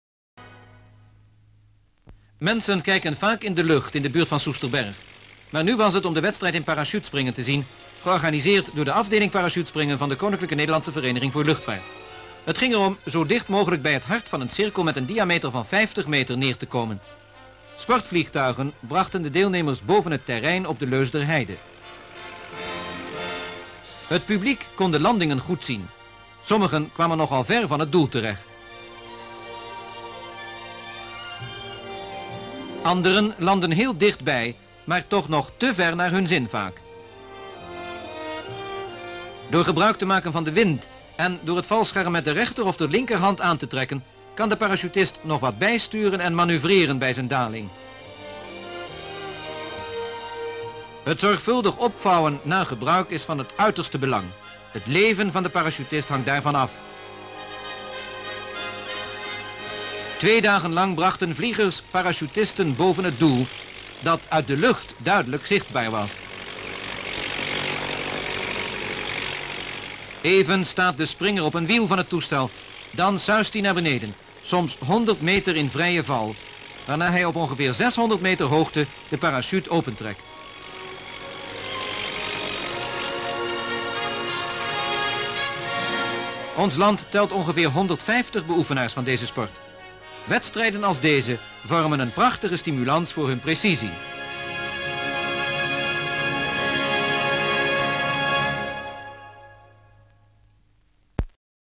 Parachute jumping (Weeknummer 67-17) 🔴
Description (more): Contest parachuting at the Leusderheide, organized by the unit of parachute jumping from KNVvL. Paratroopers go with a sports plane in the air and jump to a circle on the Heath.